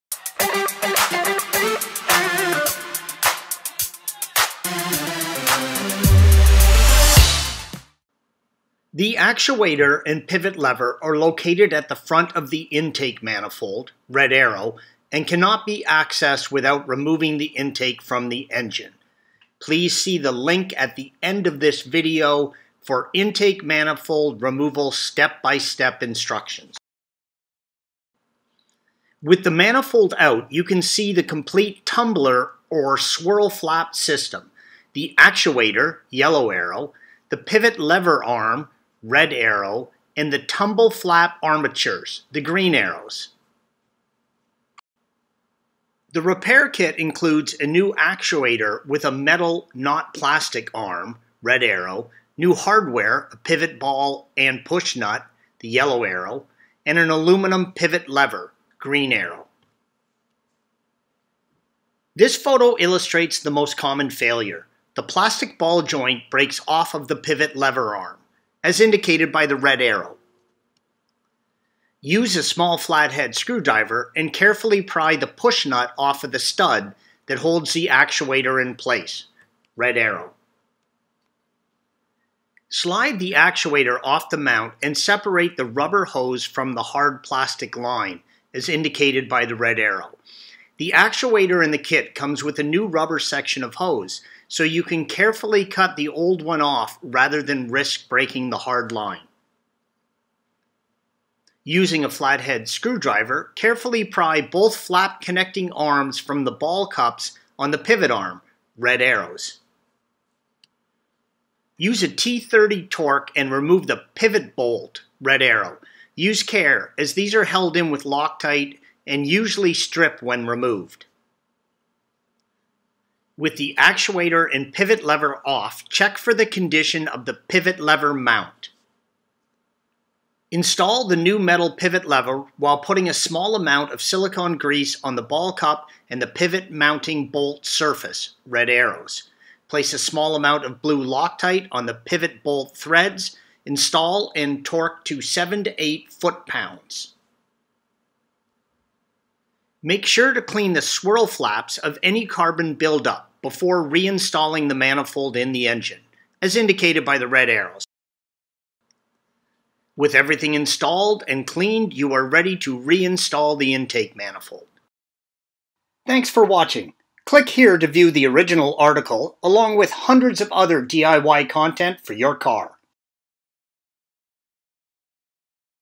Mercedes-Benz W204 C-Class Tumble Flap Actuator Repair (2008-2014)
The Mercedes C350 has a variable resonance intake manifold. This manifold can switch over from long intake runners to short intake runners by utilizing switch flaps and tumbler or flap shafts. There are two shafts, one on each side of the intake manifold. If you are getting error codes from the variable system, one of the first things you should check is the tumbler flap sensor for failure.

Both sensors are located at the rear of the intake manifold. While replacement in theory is easy I have never been able to simply access the T30 Torx screw that holds the sensor in place and remove it without having to move the wiring harness and fuel rail. If you have small enough hands that you can access the sensors then you do not need to remove anything. If you have hands larger than a ten year old you may need to remove other components.

Hot Tip: Be prepared to move things out of the way
Performance Gain:  Working variable resonance intake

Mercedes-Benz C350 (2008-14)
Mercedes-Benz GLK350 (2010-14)